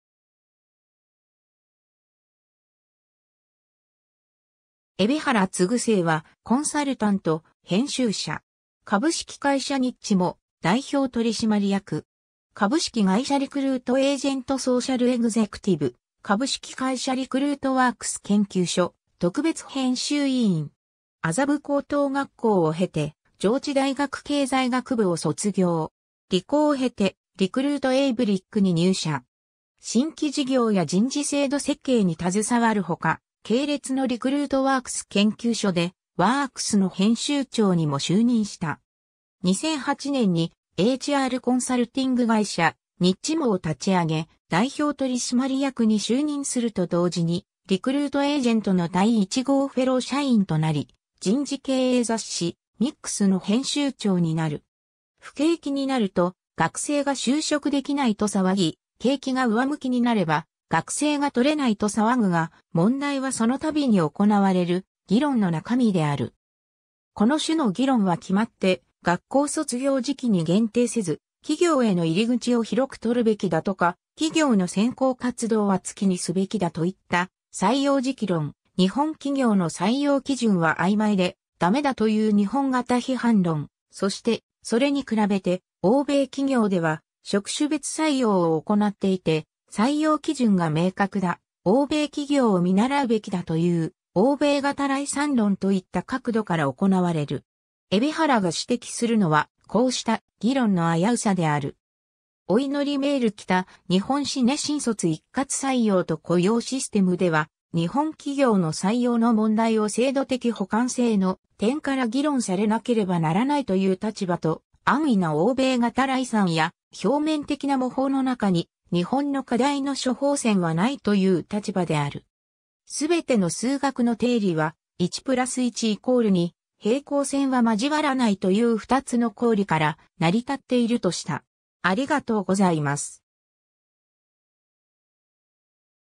海老原嗣生
海老原 嗣生（えびはら つぐお、1964年11月29日 - ）は、コンサルタント、編集者。
株式会社ニッチモ代表取締役。
株式会社リクルートエージェント　ソーシャルエグゼクティブ、株式会社リクルートワークス研究所特別編集委員。
 麻布高等学校を経て、上智大学経済学部を卒業。
リコーを経て、リクルートエイブリックに入社。
新規事業や人事制度設計に携わるほか、系列のリクルート ワークス研究所で『 Works 』（隔月刊。1995年 - ）の編集長にも就任した。
2008年に、HRコンサルティング会社、ニッチモを立ち上げ、 代表取締役に就任すると同時に、リクルートエージェントの第1号フェロー社員となり、人事経営雑誌「HRmics」（季刊誌。非売品）の編集長になる。
 不景気になると「学生が就職できない」と騒ぎ、景気が上向きになれば「学生が採れない」と騒ぐが、問題はその度に行われる議論の中身である。
この種の議論はきまって、「学校卒業時期に限定せず、企業への入り口を広くとるべきだ」とか「企業の選考活動は◯◯月にすべきだ」といった採用時期論、「日本企業の採用基準は曖昧でダメだ」という日本型批判論、そして「それに比べて欧米企業では、職種別採用を行っていて採用基準が明確だ」「欧米企業を見習うべきだ」という欧米型礼賛論といった角度から行われる。
海老原が指摘するのは、こうした議論の危うさである。
『お祈りメール来た、日本死ね　新卒一括採用と雇用システム』では日本企業の採用の問題を制度的補完性の点から議論されなければならないという立場と、安易な欧米型礼賛や表面的な模倣の中に日本の課題の処方箋はないという立場である。
 全ての数学の定理は「1+1=2」「平行線は交わらない」という２つの公理から成り立っているとした。